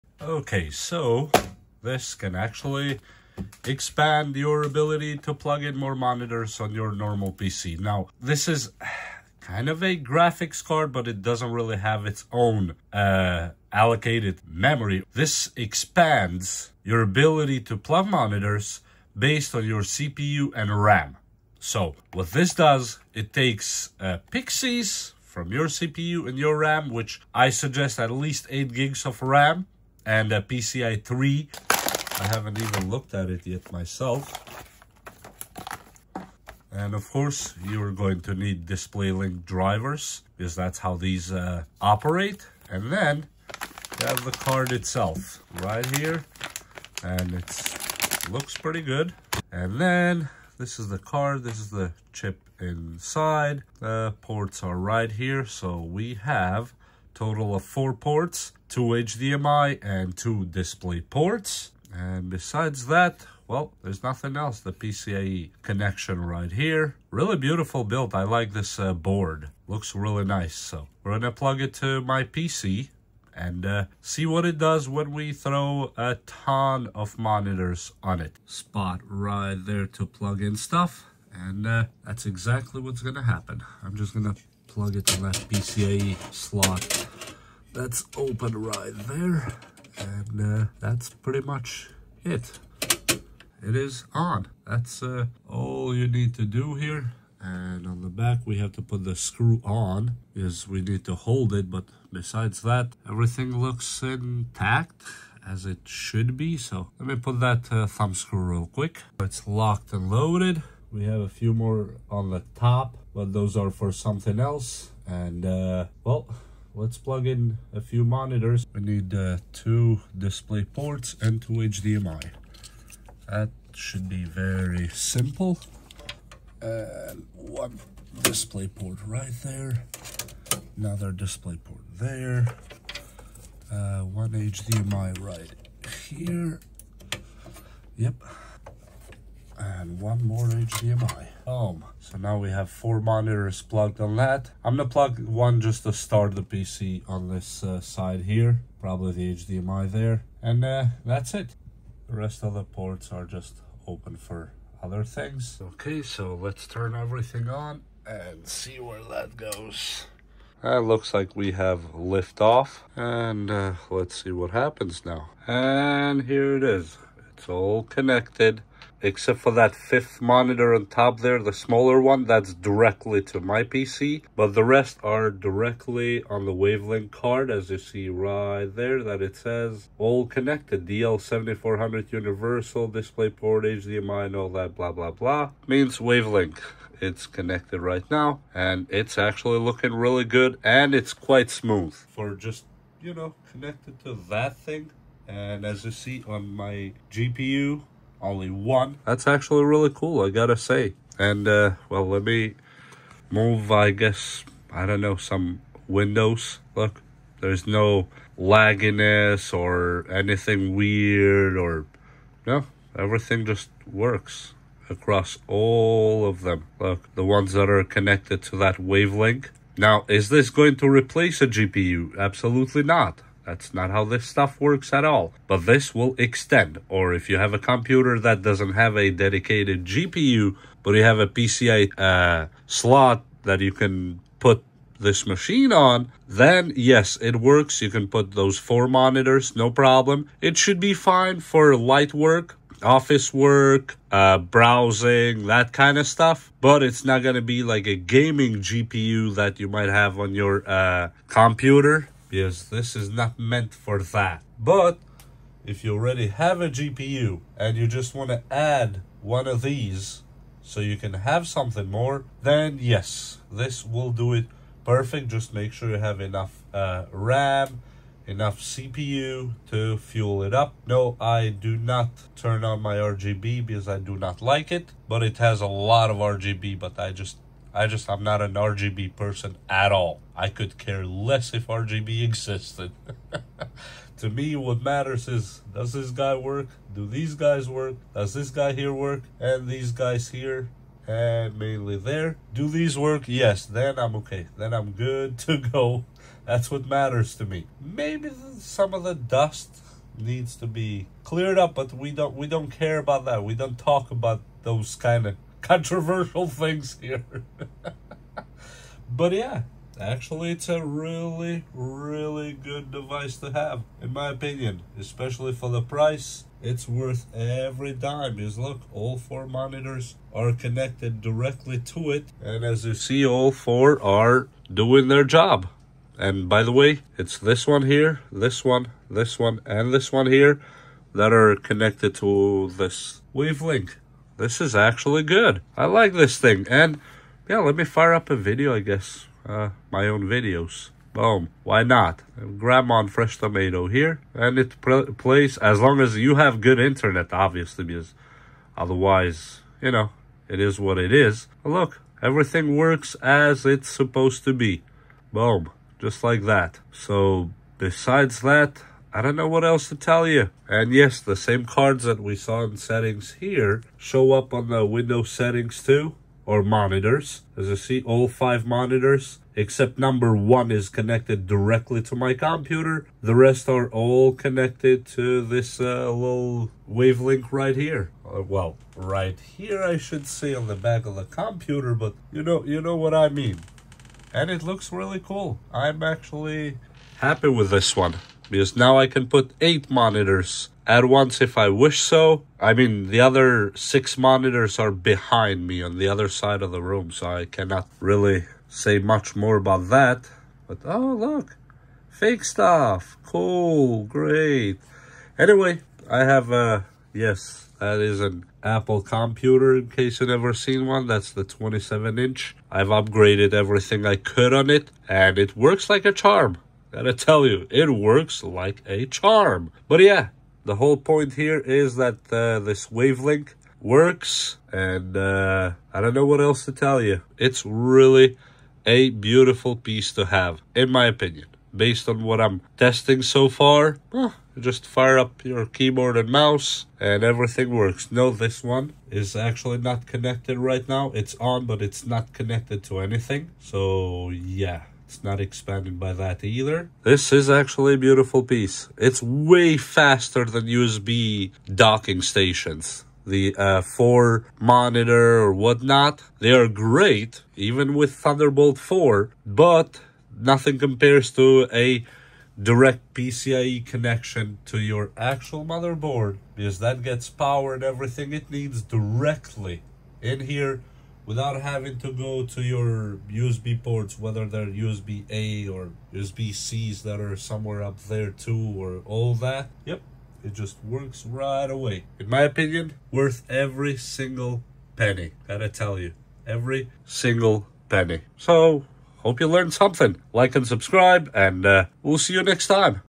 Add 4 Extra Monitors to Any PC! (WAVLINK 8K/4K Display Card Review)
💻 Want more screen real estate for trading, coding, editing, or productivity? In this video, I review the WAVLINK PCIe 8K/4K Multi-Monitor Card that adds up to 4 extra monitors (2x DisplayPort + 2x HDMI) to your Windows 11, Windows 10, or Ubuntu system.